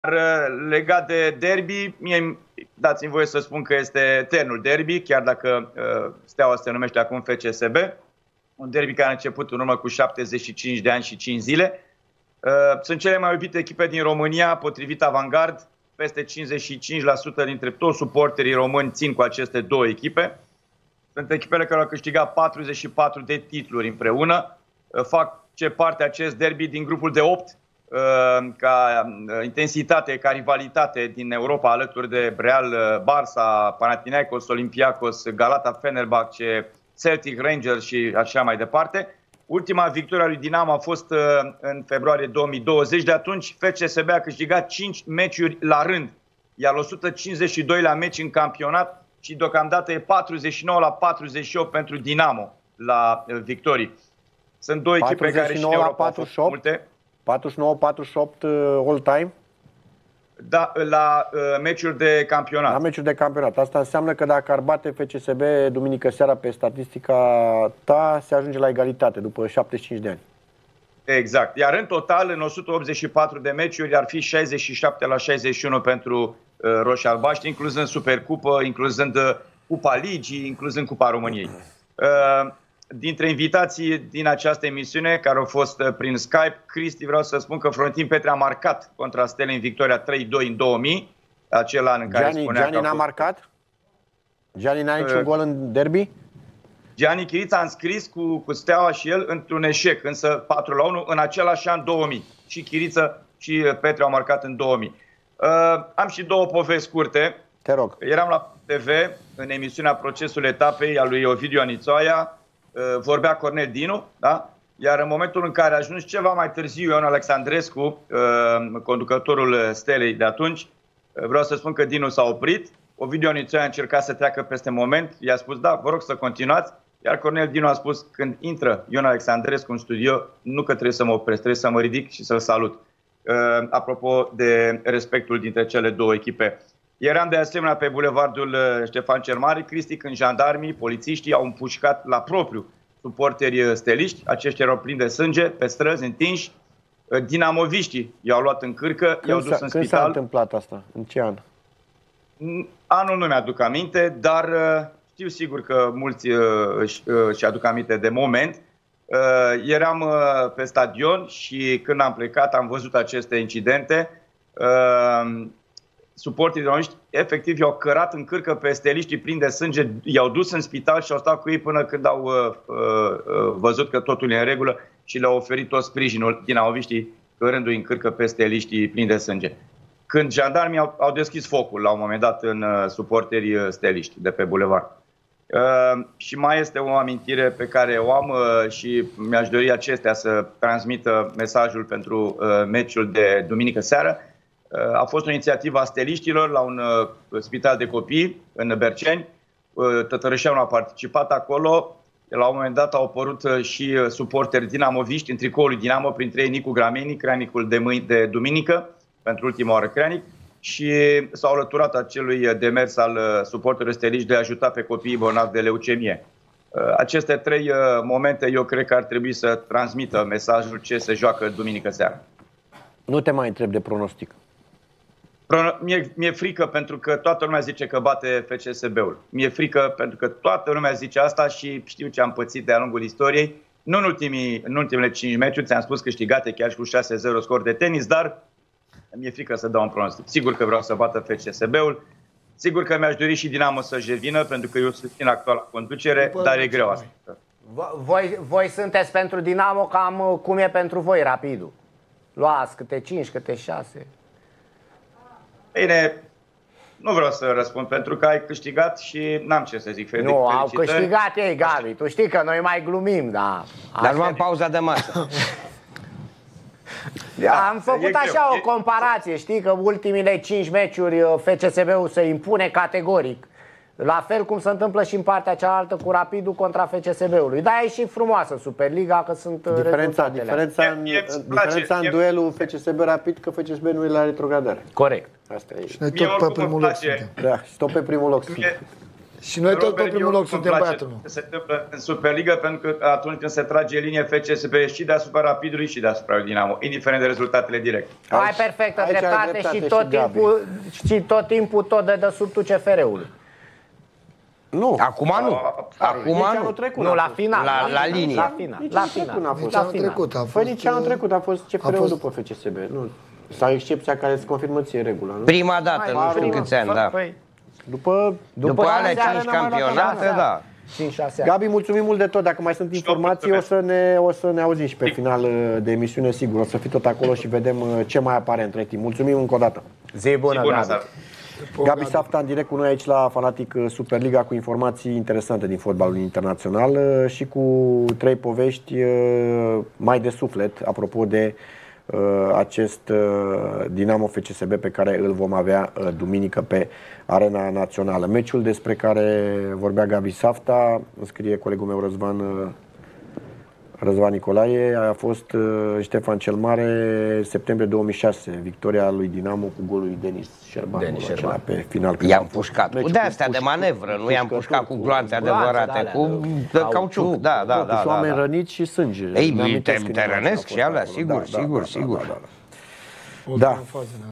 Gabi Safta, AMINTIRI CU IMPUSCATURI de la Dinamo - Steaua
Gabi Safta a vorbit la FANATIK SuperLiga despre un derby Dinamo - Steaua in care suporterii stelisti au fost impuscati de jandarmi. Moment incredibil din istoria derby-urilor Dinamo - Steaua.